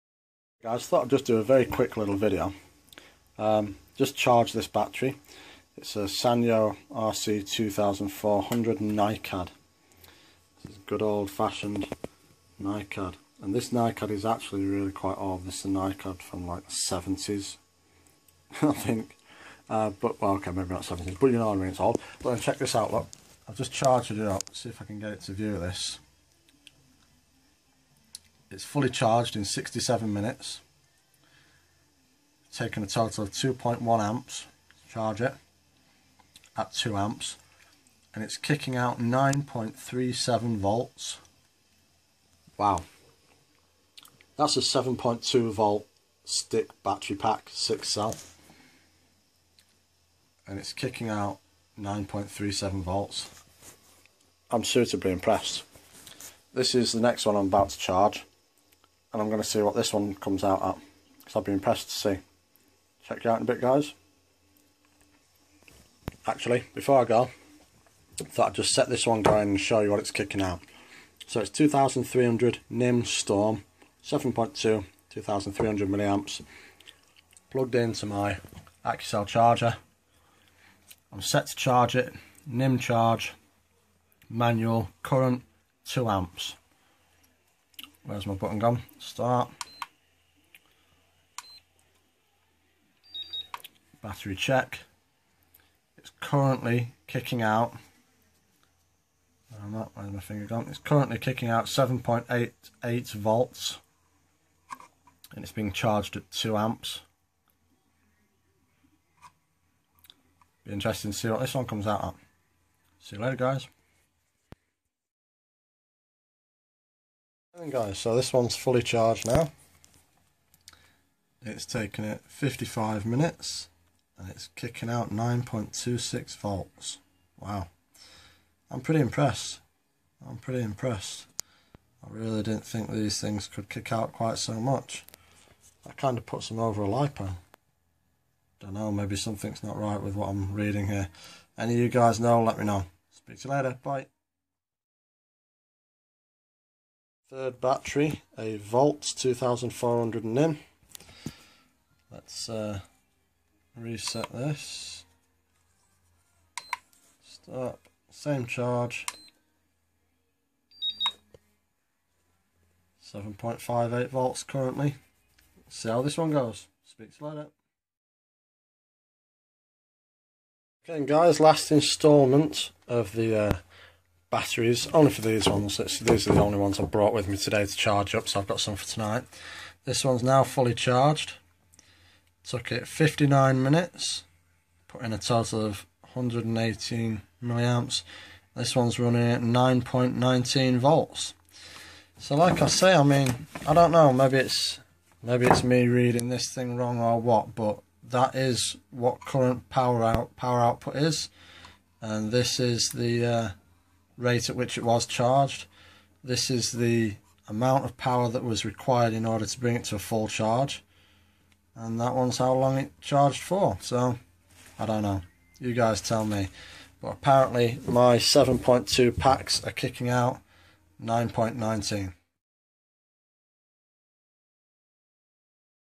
7.2v stick battery output and charge cycles.
Short video of the amount of voltage my apparent 7.2v stick batteries put out.